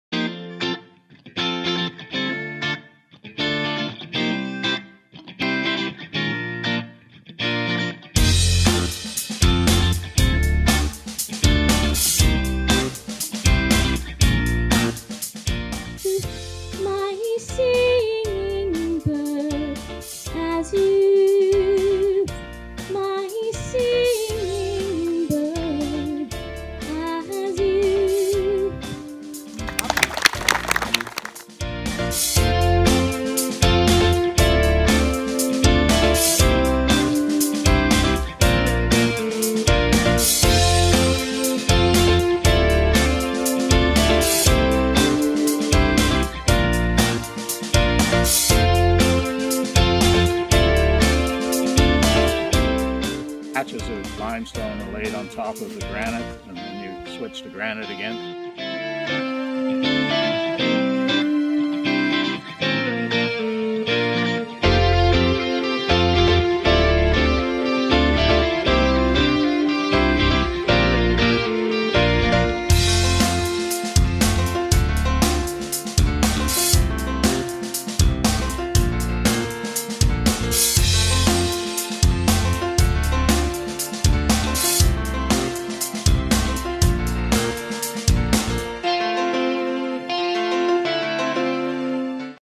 Stony Lake Trails 1 Year Anniversary Event
- KAWARTHA LAND TRUST -
protecting the land you love.

Thank you to all of those who came out and attended our July 8th, 2017 event. Stony Lake Trails is officially 1 year old!